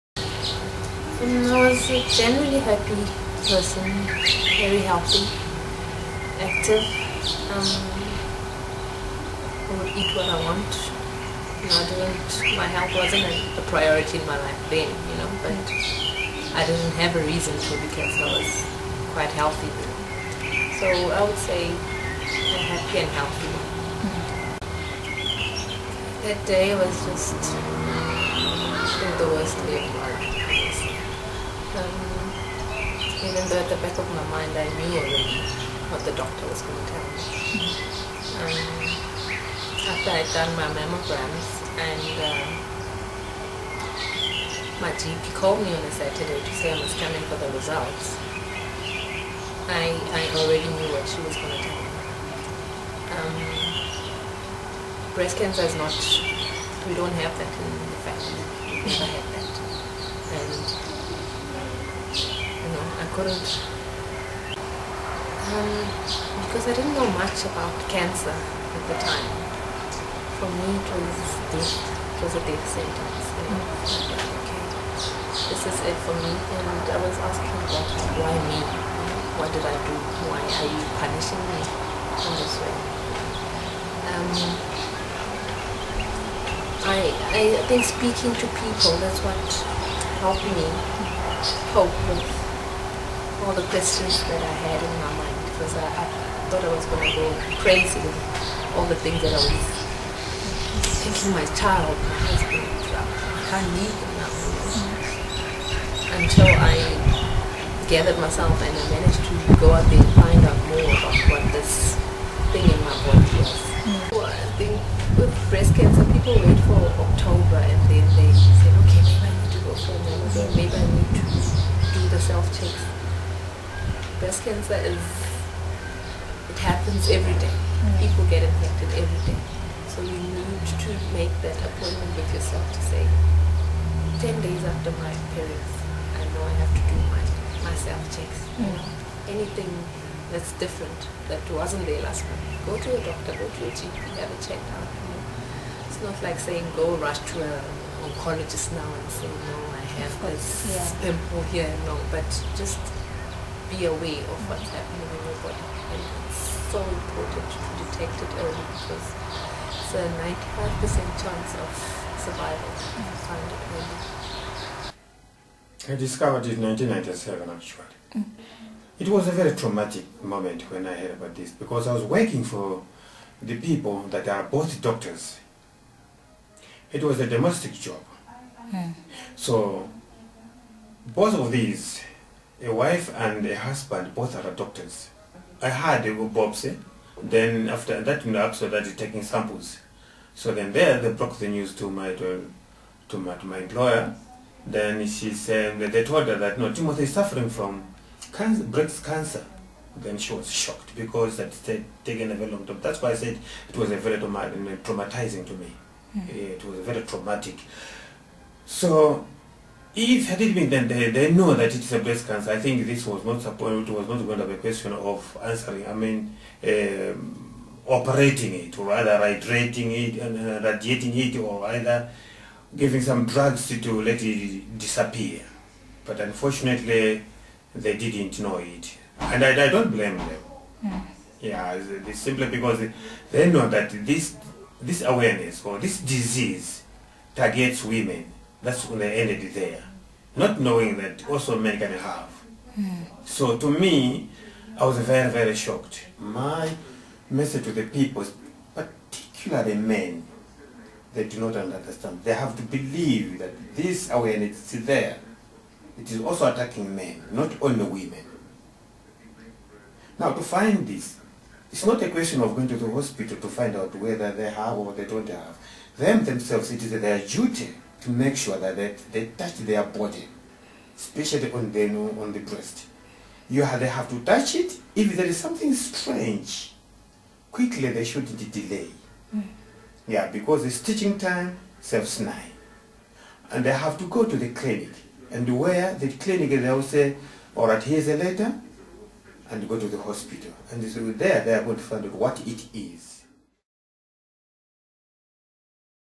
Breast cancer survivors speak out
Breast cancer survivors, Rebecca Baloyi, wife of footballer, Brian Baloyi and Timothy Mlenje talk about their fight against cancer and how it changed their lives.